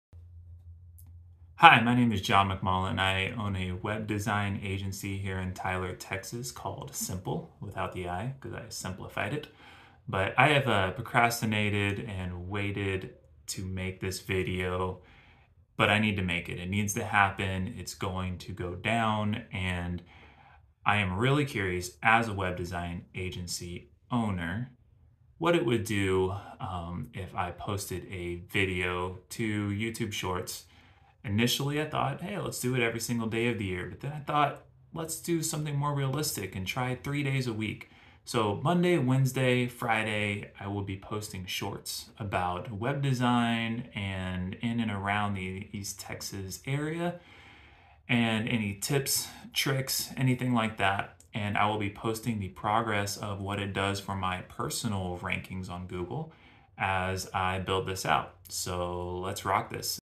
Starting YouTube Shorts as a Tyler Texas Web Design Agency
Video #1 in a series of YouTube shorts aimed at helping business owners discover what works with marketing your business on Google.

Looking to ramp up your online presence in 2023? We'd love to talk to you!

We're here for your Tyler Texas web design needs!